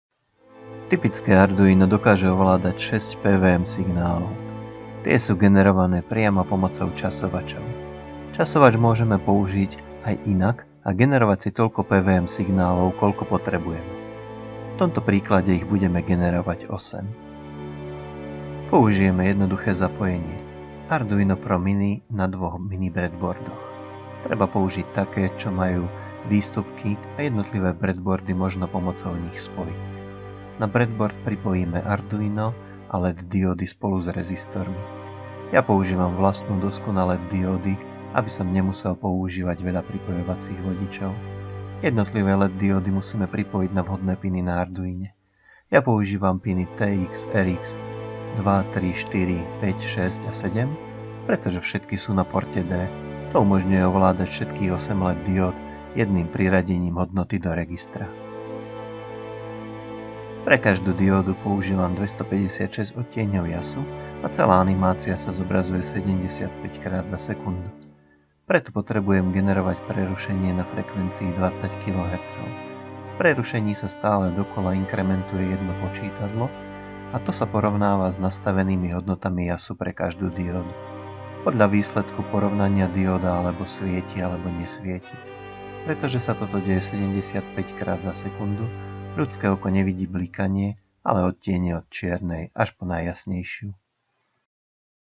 8x PWM from Arduino
PWM signal is created using CTC mode of Timer1 and interrupt. Frequency is set to 20 kHz, because I need 75 frames/second and 256 shades of color.

USB to TTL Serial Converter

Mini Breadboard